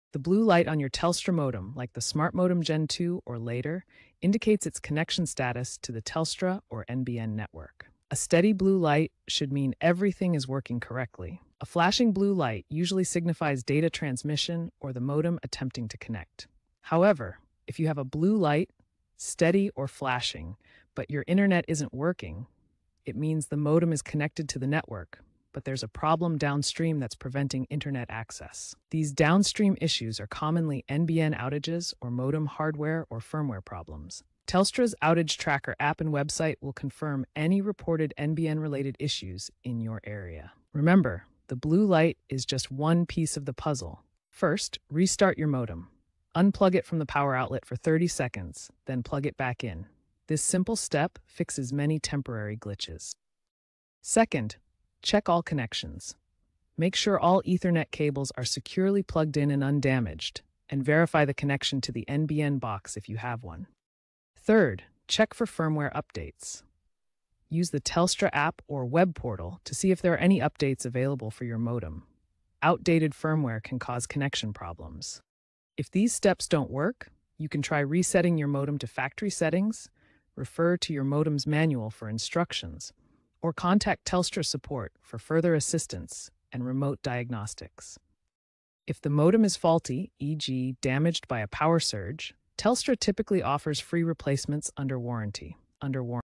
How to Fix Blue Light on Telstra Modem – Meaning, Internet Down & Fixes
If your Telstra modem has a blue light, find out what it means and how to fix it. Includes internet outage checks and simple resets.